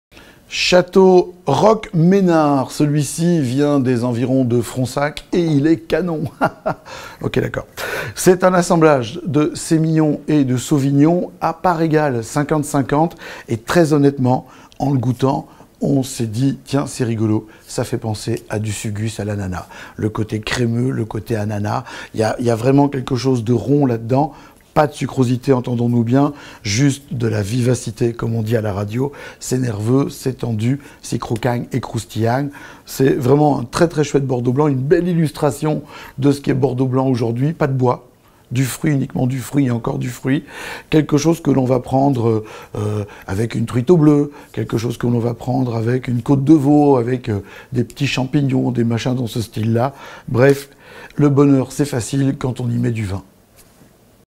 Château Roc Meynard Blanc 2016 – Bordeaux
Eric Boschman présente le vin de Bordeaux Château Roc Meynard Blanc 2016 sélectionné par le comité de dégustation de Vinogusto.